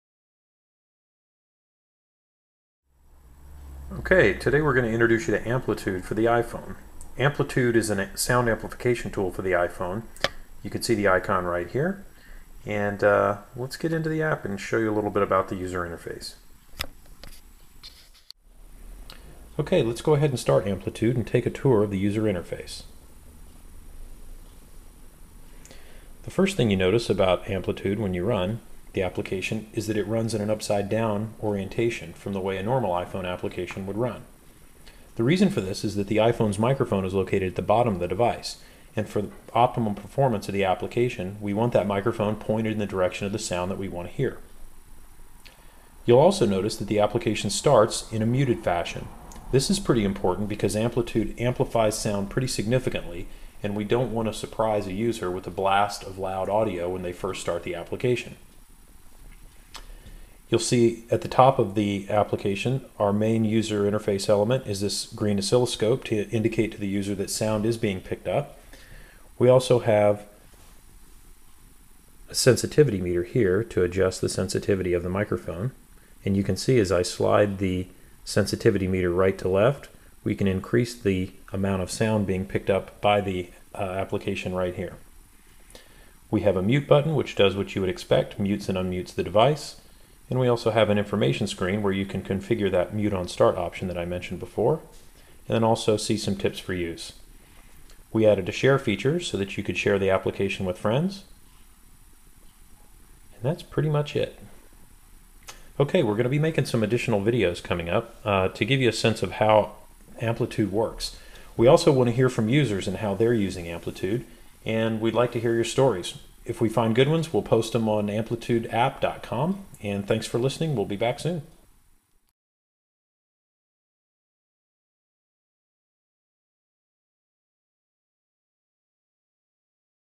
Amplitude - Intro
This is a brief overview of Amplitude for iPhone.  In this video we provide an overview of the user interface and explain what each element in the UI does.